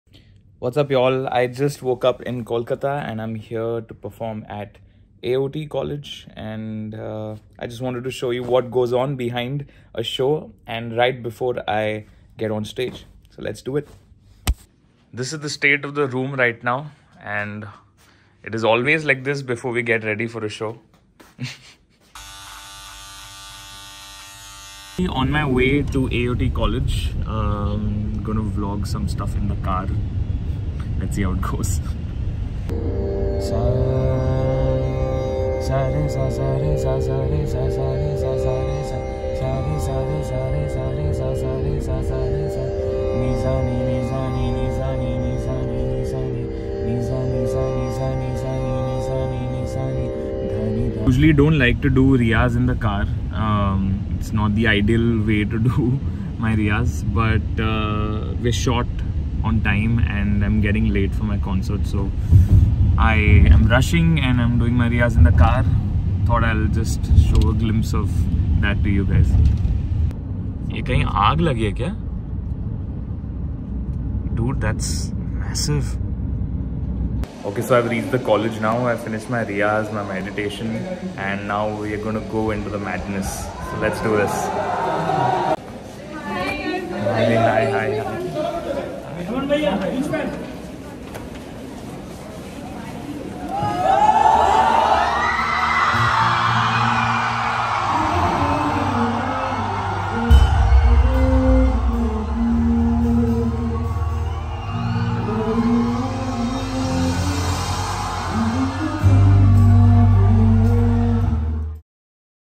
Show ke pehle ka vlog | Armaan Malik
Yay, I made a longer vlog! Longer by a minute lol. This was me getting ready before stepping onstage at AOT College (West Bengal).